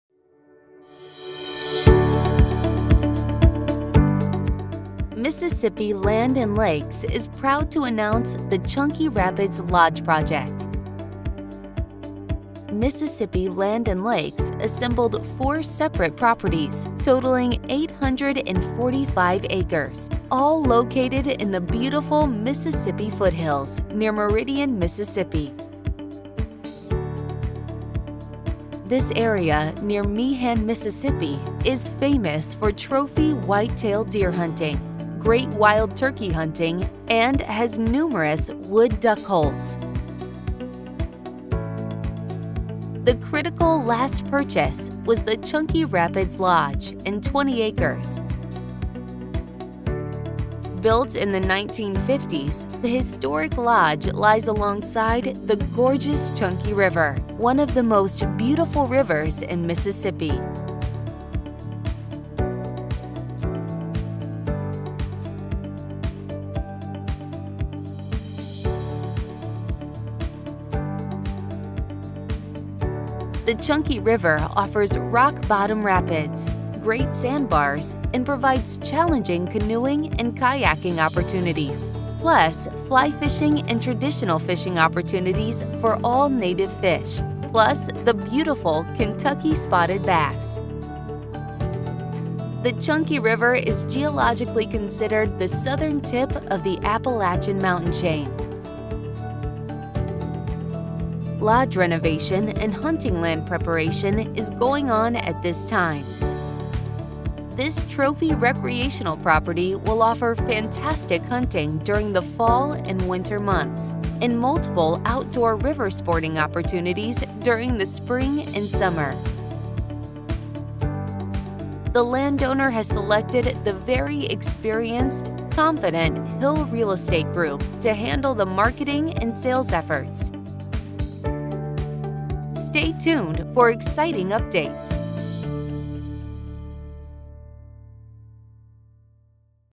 Land and Lakes Chunky Rapids
Mississippi Land and Lakes is proud to announce  the Chunky Rapids Lodge Project.  Mississippi Land and Lakes assembled 4 separate properties totaling 845 acres all located in the beautiful Mississippi Foothills near Meridian, MS

This area near Meehan, MS is famous for trophy whitetail deer hunting, great wild turkey hunting and has numerous wood duck holes.  The critical last purchase was the Chunky Rapids Lodge and 20 acres.  Built in the 1950's the historic lodge lies alongside the gorgeous Chunky River, one of the most beautiful rivers in Mississippi. The Chunky River offers rock-bottom rapids, great sandbars and provides challenging canoeing and kayaking opportunities plus fly fishing and traditional fishing opportunities for all native fish plus the beautiful Kentucky spotted bass.

The Chunky River is geologically considered the southern tip of the Appalachian Mountain chain.  Lodge renovation and hunting land preparation is going on at this time.

This trophy recreational property will offer fantastic hunting during the fall and winter months and multiple outdoor river sporting opportunities during the spring and summer.

The landowner has selected the very experienced competent Hill Real Estate Group to handle the marketing and sales efforts.